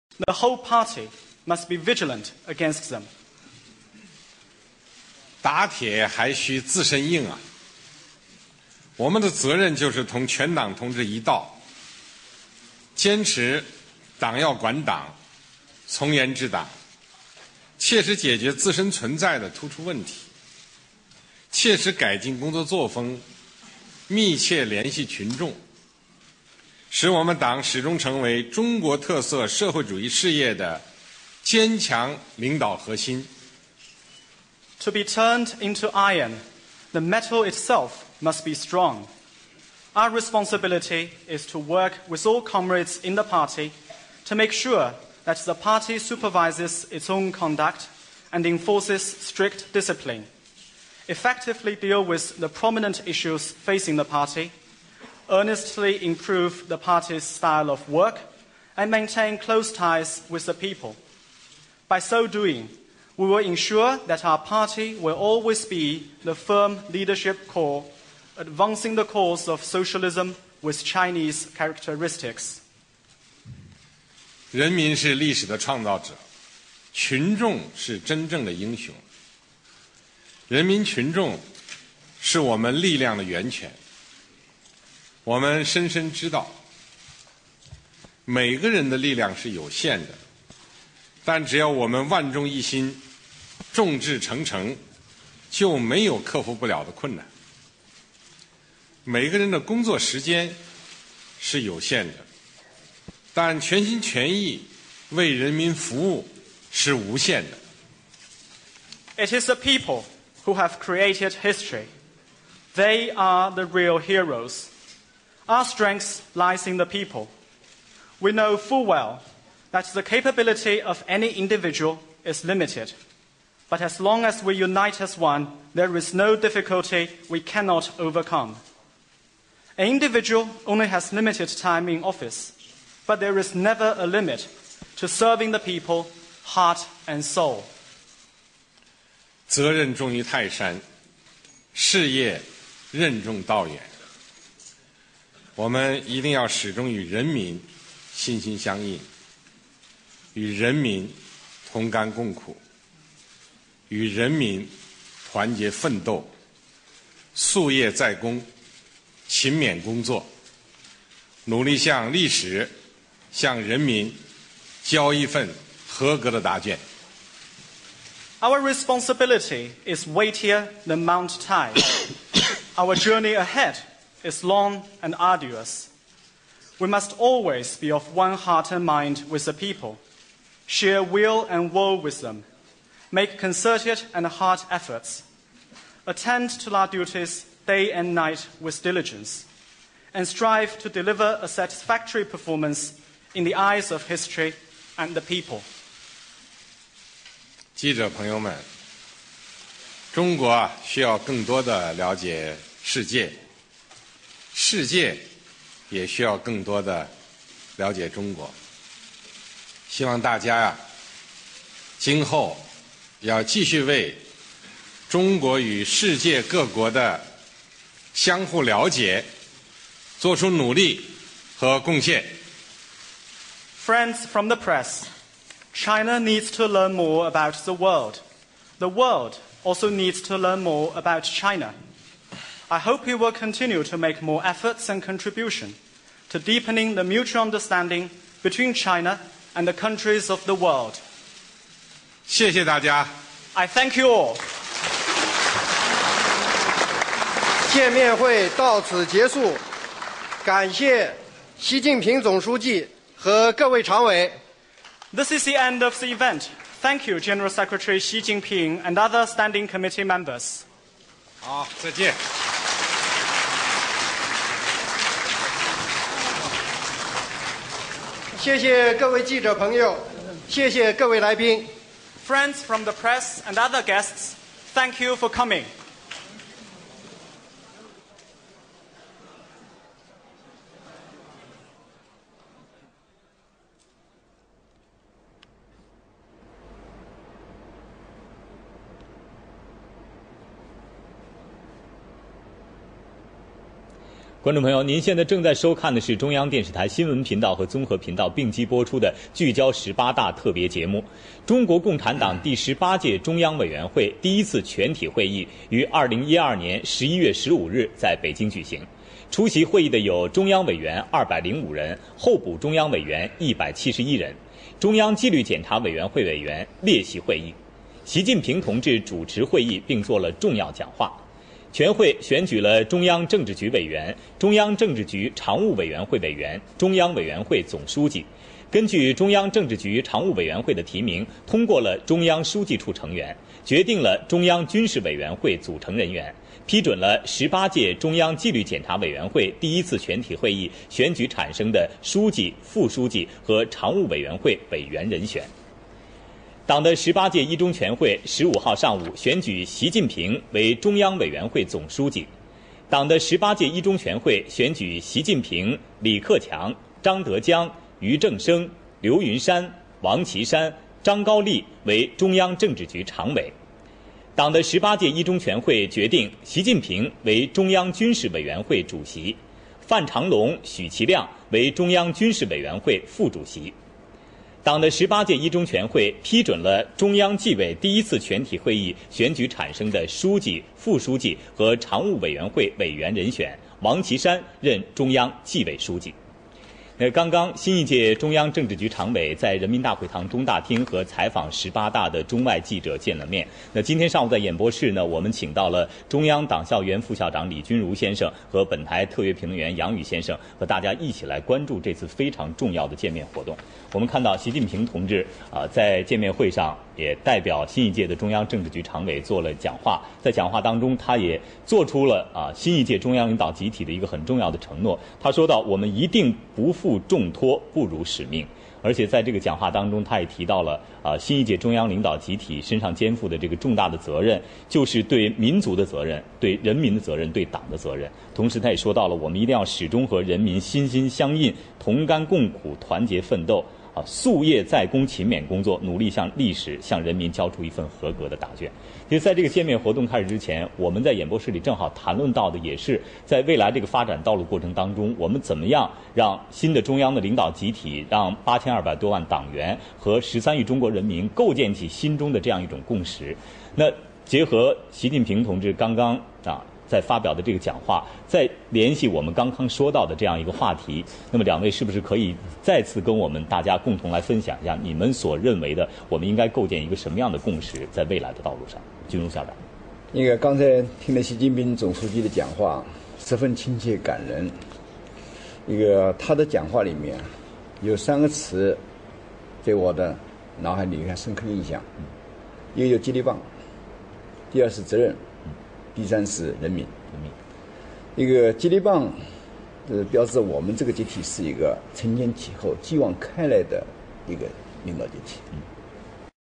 习近平上任后的第一次公开讲话 （2）
2012年11月15日习近平在常委见面会上的讲话 （2）